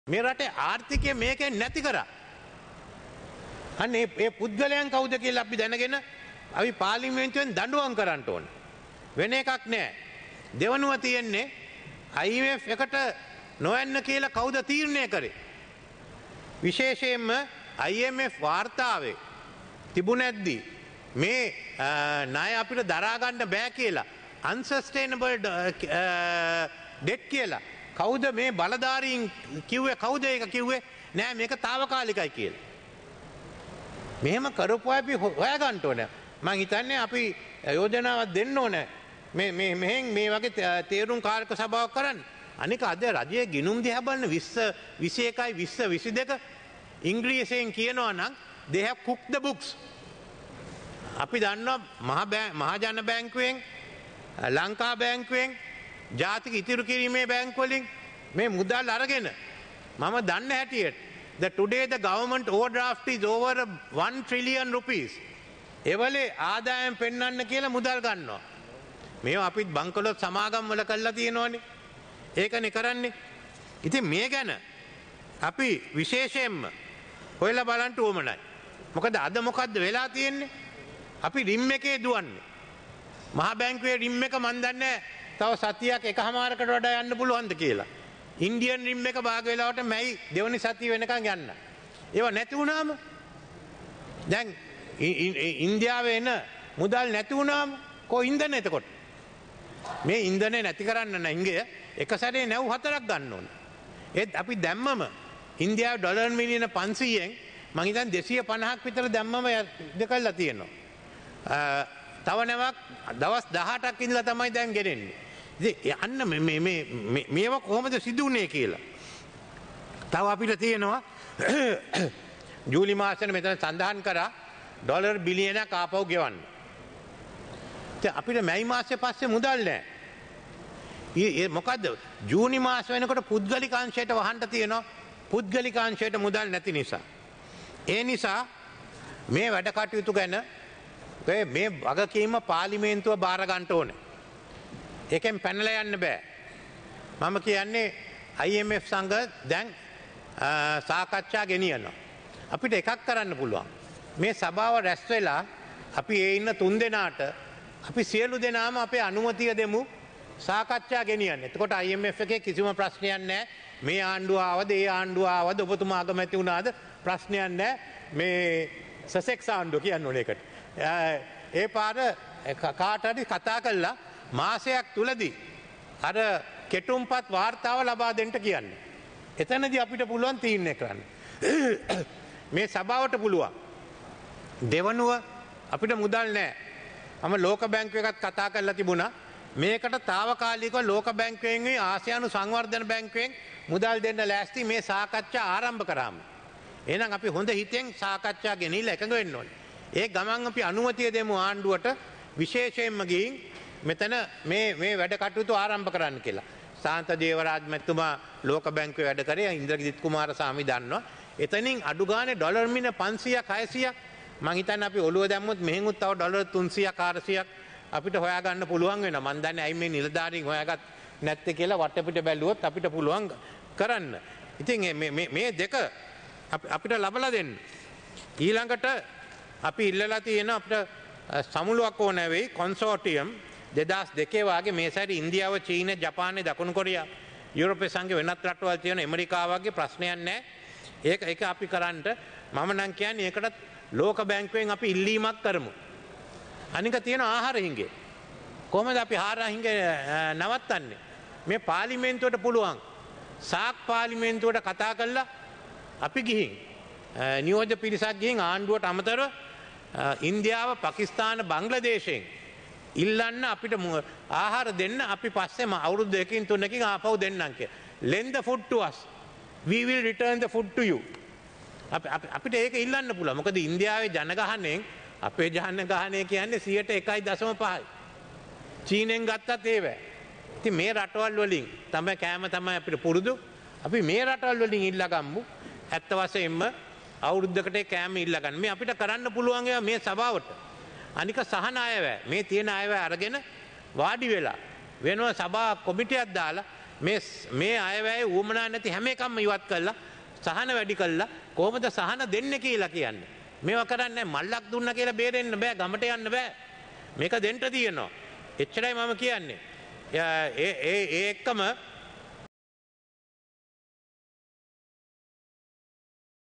ඉන්දියාවෙන් පකිස්ථානයෙන් ආහාර ඉල්ලා ගමු ආපසු දෙන්න බලාගෙන -රනිල් වික්‍රමසිංහ | Breaking.lk
ඉන්දියාවෙන් පකිස්ථානයෙන් ආහාර ඉල්ලා ගමු ආපසු දෙන්න බලාගෙන  -රනිල් වික්‍රමසිංහ